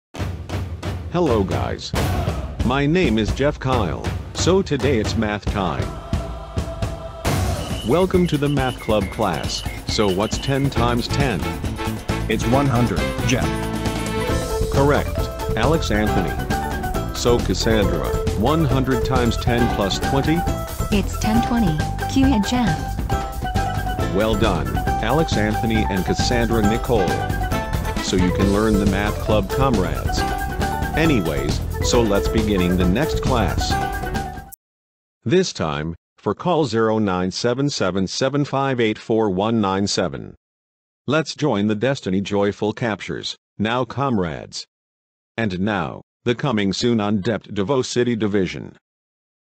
JeffKyleMedia Math Club TV Commercial (2024)
This Educational Math Club, for the JeffKyleMedia Math Club, Coming Soon on The DepED Davao City Division for the Rest of Future!

Well, Having Watching some More Vyond Videos, Movies, Gaming Videos, Adventure Videos, and More!

Programs Used:
Camtasia 2024
StreamElements TTS
Windows 10